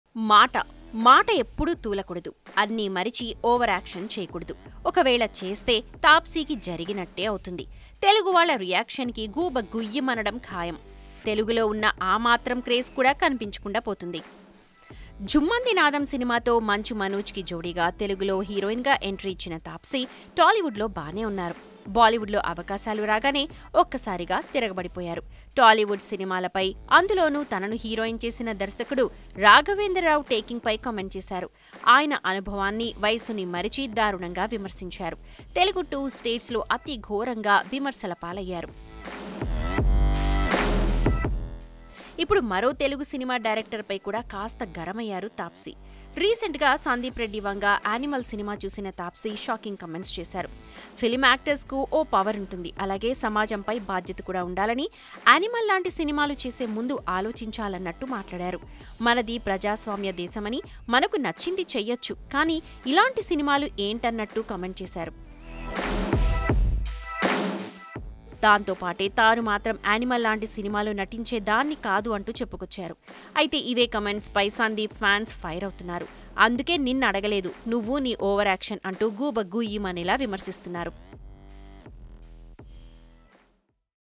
గూబ గుయ్‌ మంది..! ఓవర్ యాక్షన్ చేస్తే.. గిట్లే ఉంటది మరి.. | Taapsee Pannu - TV9 ET
మాట..! మాట ఎప్పుడూ తూలకూడదు..! అన్నీ మరిచి.. ఓవర్ యాక్షన్ చేయకూడదు..! ఒకవేళ చేస్తే.. తాప్సీకి జరిగినట్టే అవుతుంది. తెలుగు వాళ్ల రియాక్షన్‌కి గూబ గుయ్‌ అనడం ఖాయమైతది. తెలుగులో ఉన్న ఆ మాత్రం క్రేజ్‌ కూడా.. కనిపించకుండా పోతది.

Credit: entertainment |SateeshChandra/Producer